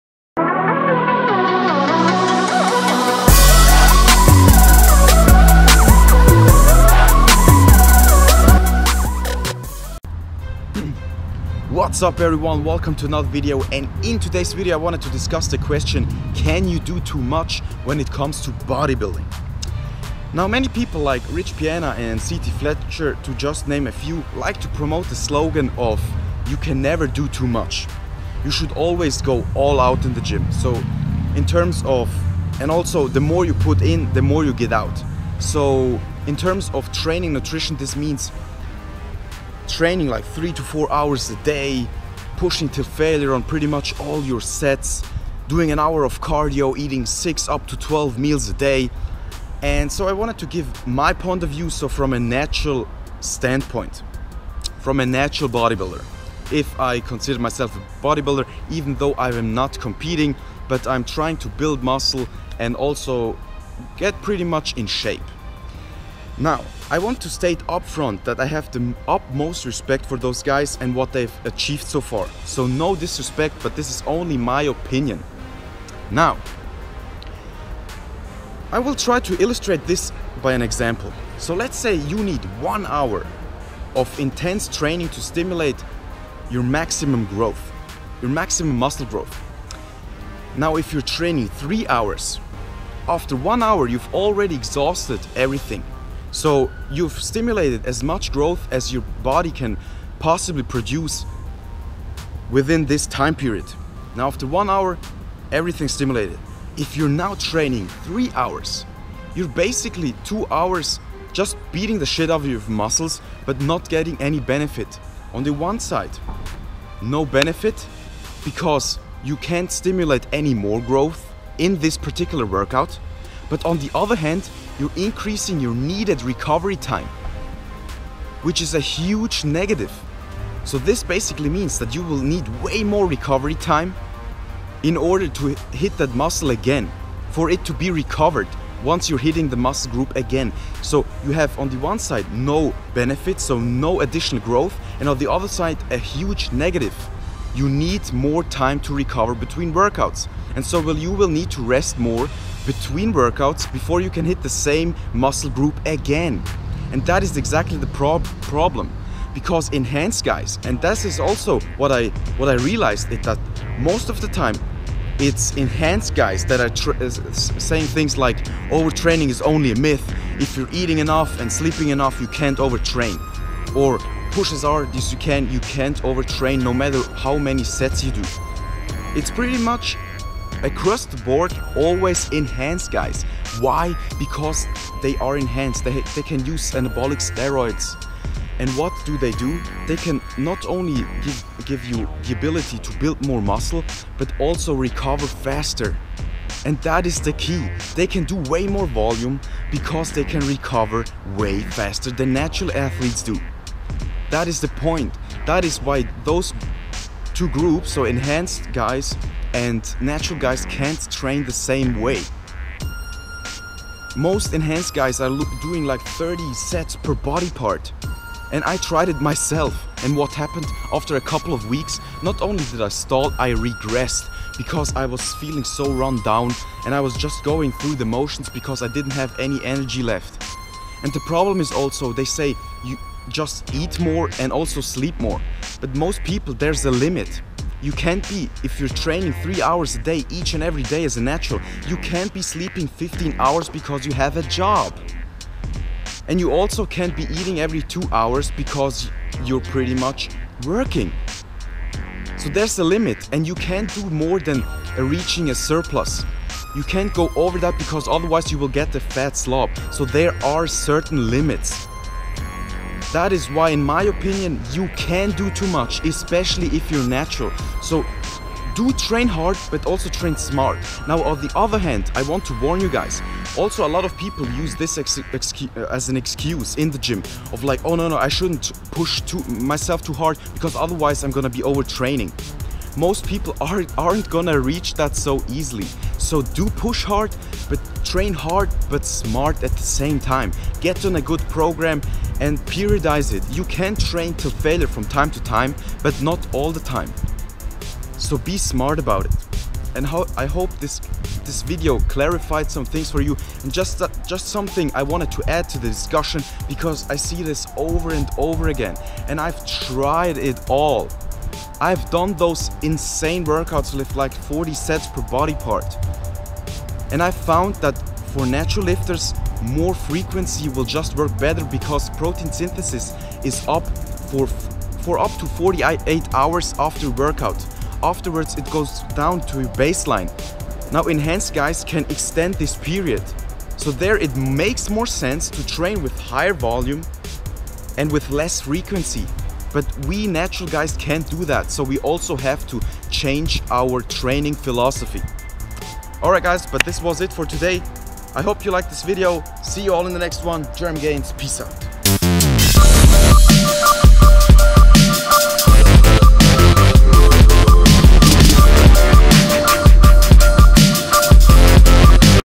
RESPONSE TO RICH PIANA: Can you do too much? 4k HD
In this video I am giving my point of view to the statement: You can't do too much in the gym!